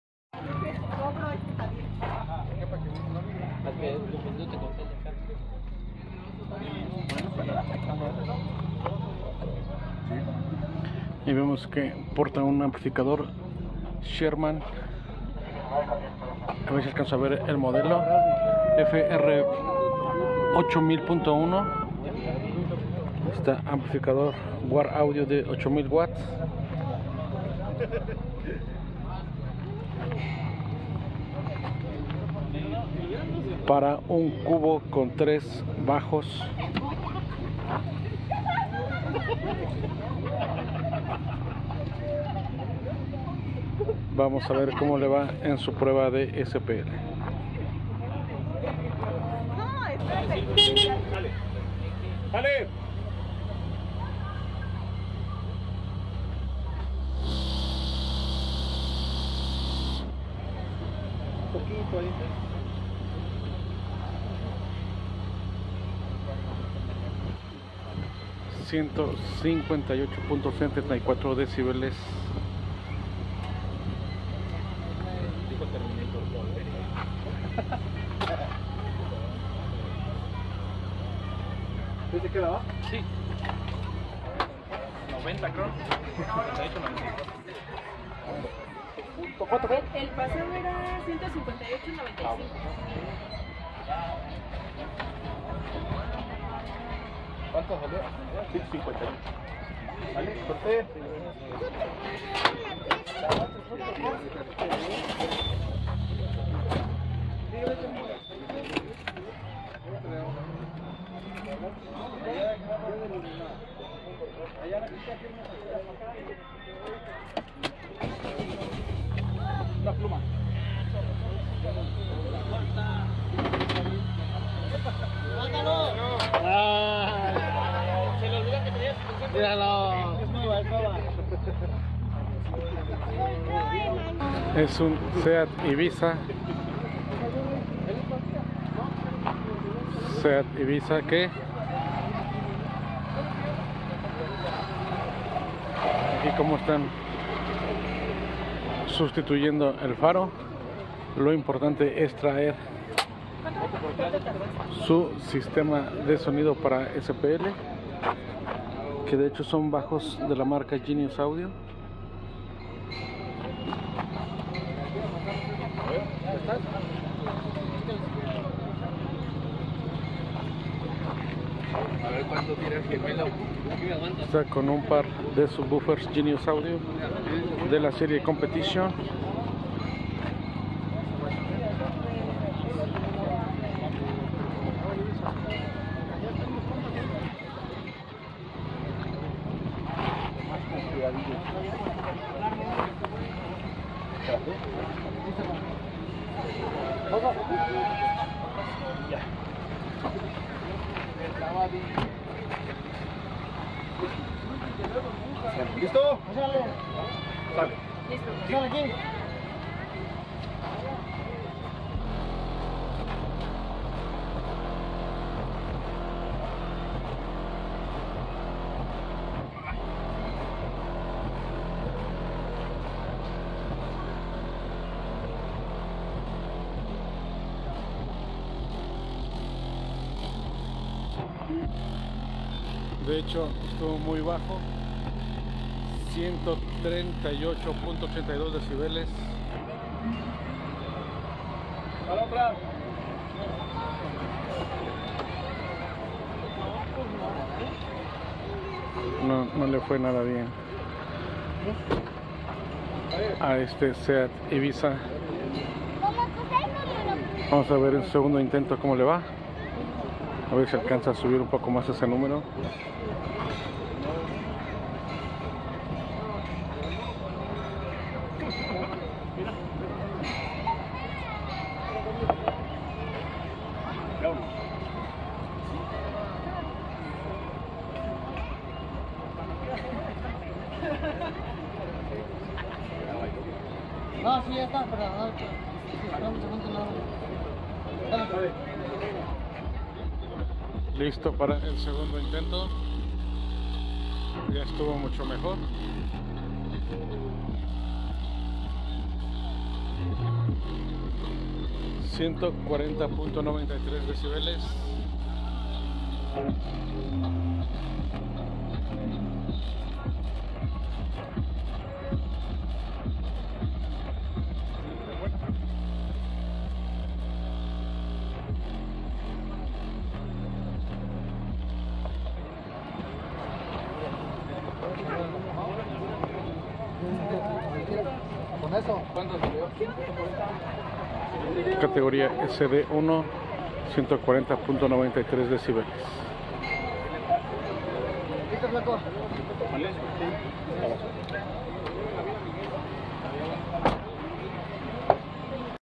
¡158.95dBs SPL con equipo War Audio!
La competencia se llevó a cabo en el Inmortal Legends realizado en Tula, Hidalgo.